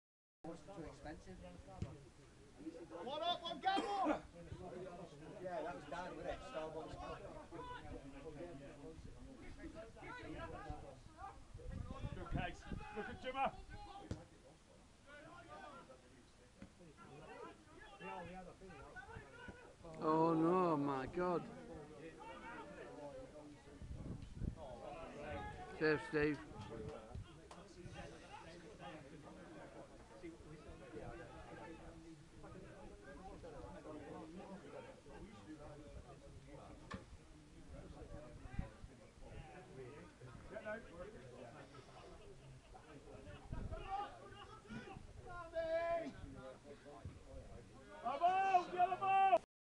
ECPL PREMIER LEAGUE GAME, St Stephen 1st 2 v 1 Gerrands/St Mawes
Match highlights, Casey Laity keeps the ball and lays it back to Luke Damerell who clears up the field the ball comes back and this time Luke Damerell messes up his clearance and Gerrand's no 11 Guest puts no 8 Young   through he races in on goal but his shot thankfully is saved by Steve Raven eventually Luke Damerell cleans up.